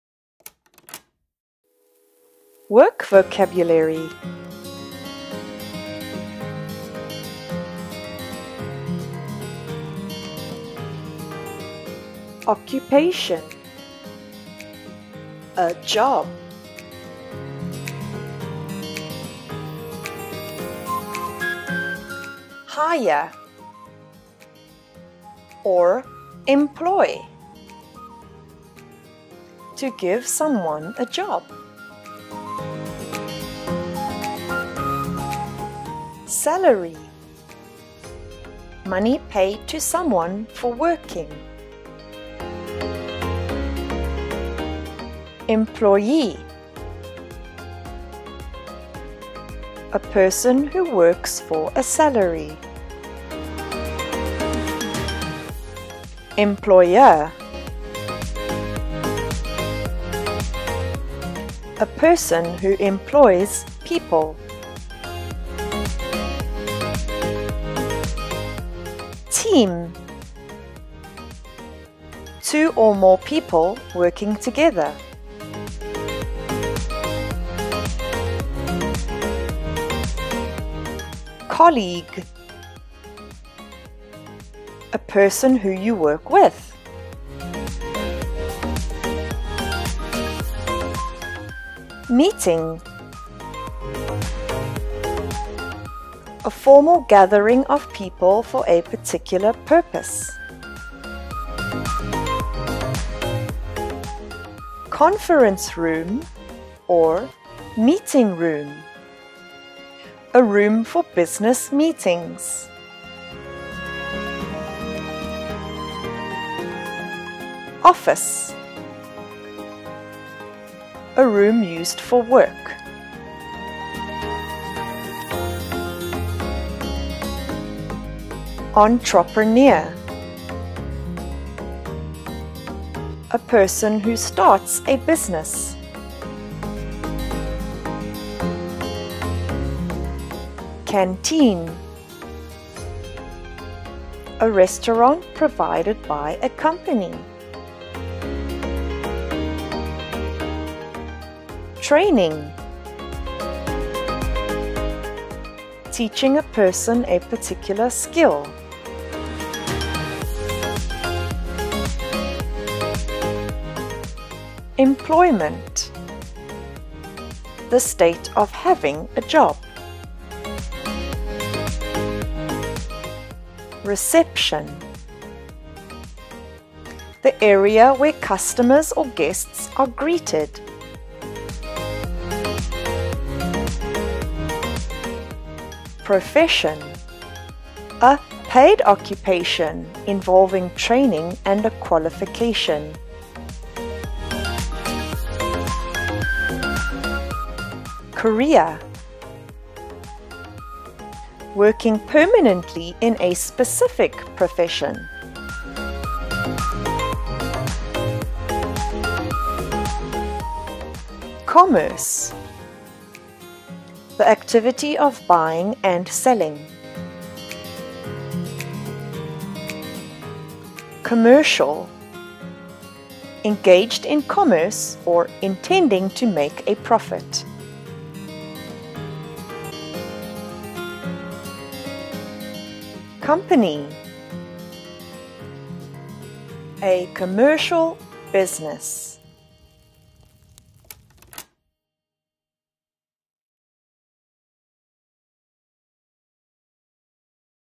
Work Vocabulary - Learn Work Related English Vocabulary
Learn new English work related vocabulary words.
Everyday useful work vocabulary.
Learn to speak English fluently.
English pronunciation and reading practice.

Edited and Produced by Rock Rabbit.